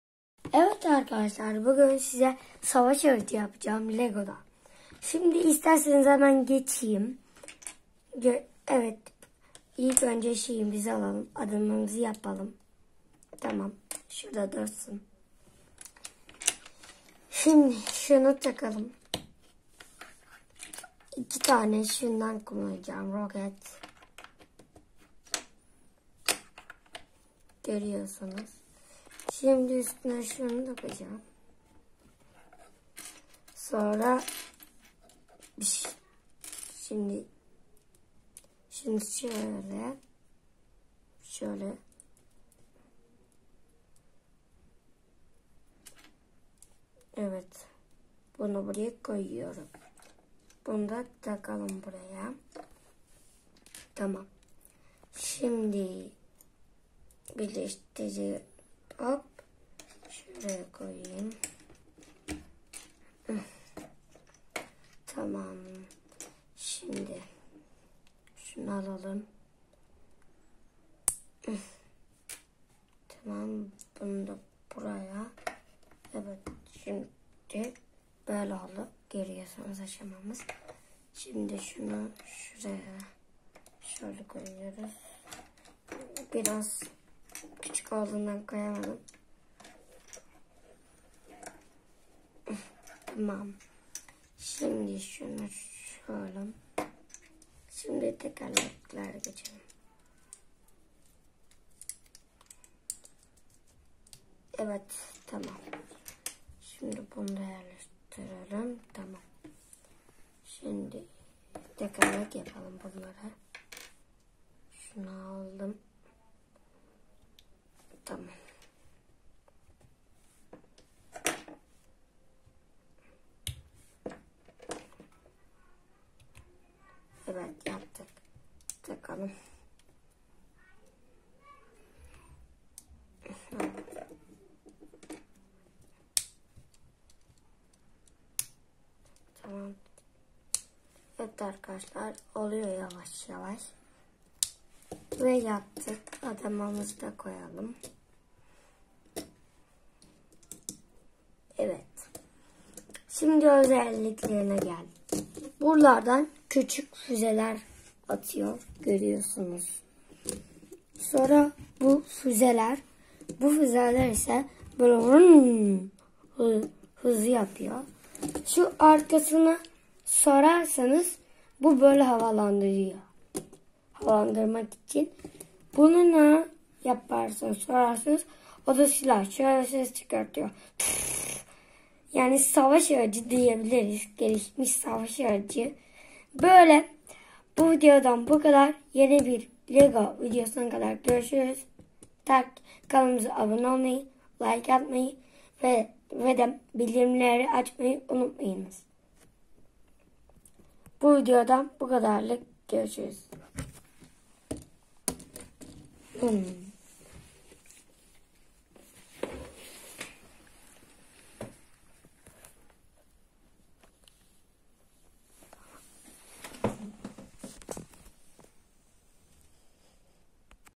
Lego Araba Tasarımı Robotik Kodlamaya Giriş - Lego Design Car
Lego Tasarım Araba Robotik Kodlamaya Giriş. Çocukların kendilerine özgü tasarım dünyaları.